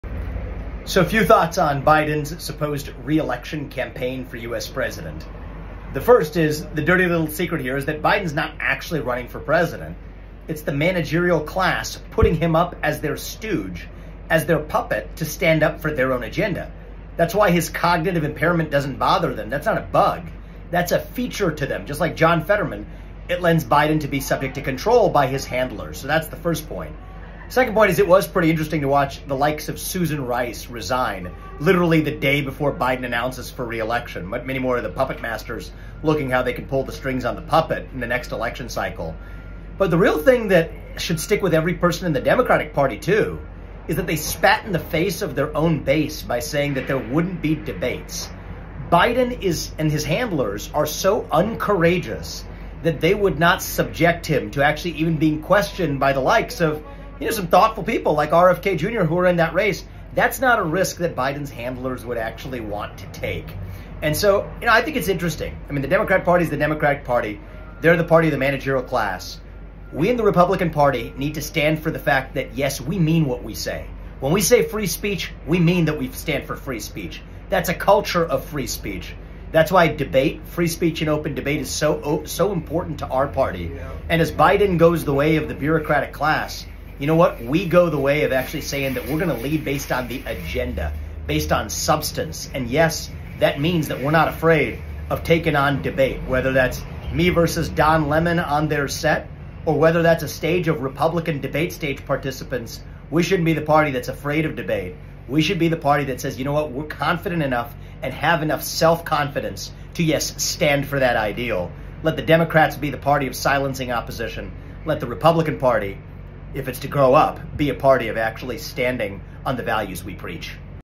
The DNC is Spitting in the Face of its Own Base: Vivek Ramaswamy Responds to the New DNC Edict
The @DNC is spitting in the face of its own base by refusing to let challengers debate Biden. The managerial class won’t allow it. We’re better than that on our side. Let’s keep it that way.

Truth Social - @VivekRamaswamy
Rumble - @VivekRamaswamy